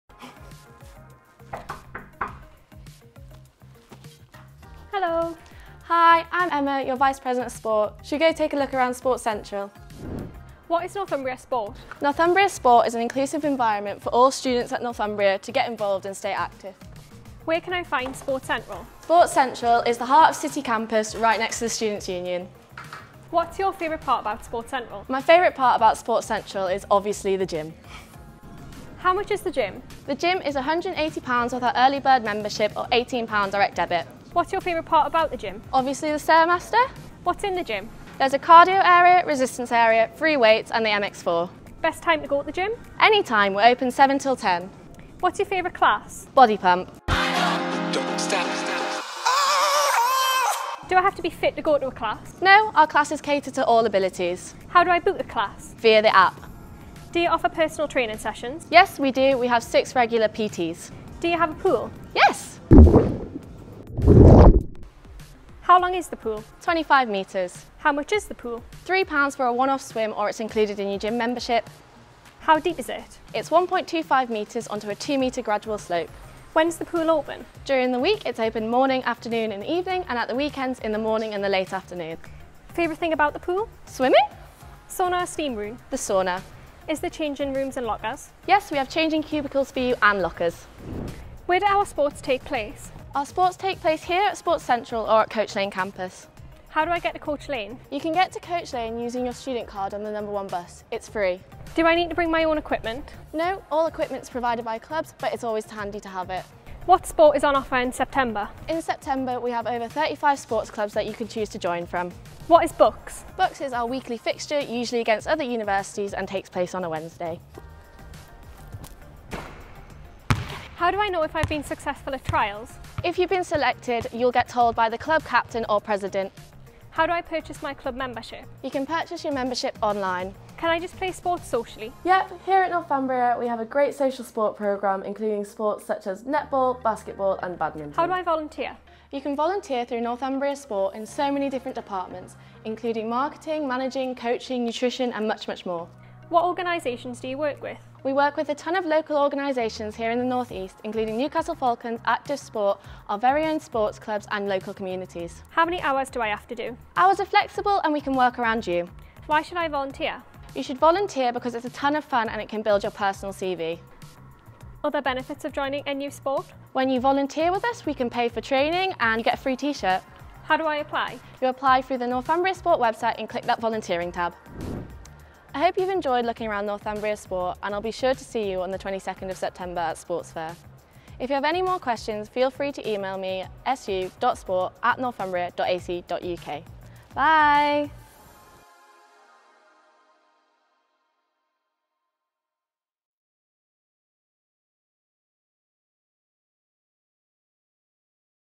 33 Questions with Vice President Sport Emma Collier
Welcome to Northumbria Sport 2021/2022. Meet your Vice President of Sport Emma Collier, you can find out everything you need to know about Sport in this video!